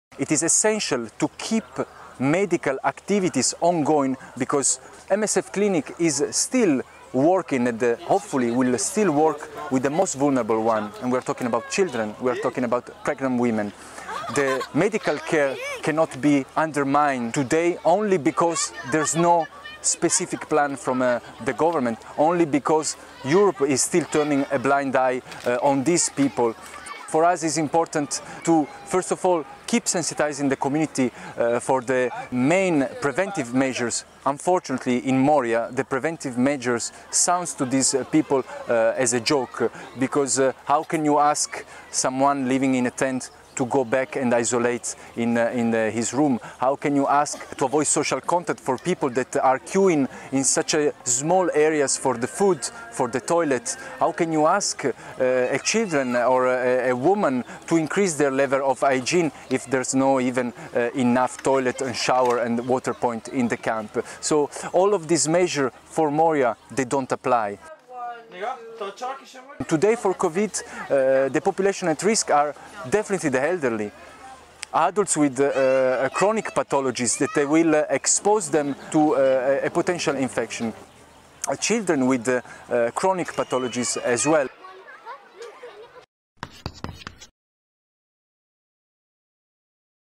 COVID-19 preparations in Moria camp, Greece
MSF is calling for the immediate transfer of the high risk population, elderly and patients with chronic diseases, living in the camps to places where all the preventive measures can be applied. Forcing people to live in overcrowded and unhygienic camps as part of Europe’s containment policy was always irresponsible but now more than ever due to the COVID-19 threat.